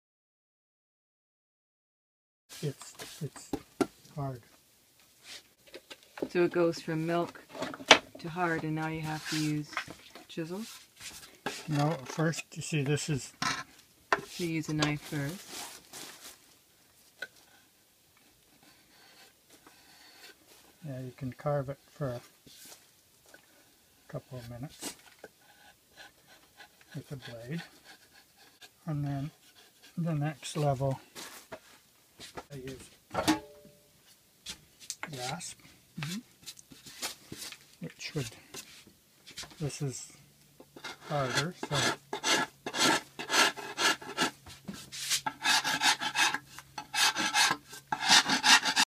Forming a hydrostone sculpture- The Rising Stone Studio
Stephen Cole showing the process of a lifecast sculpture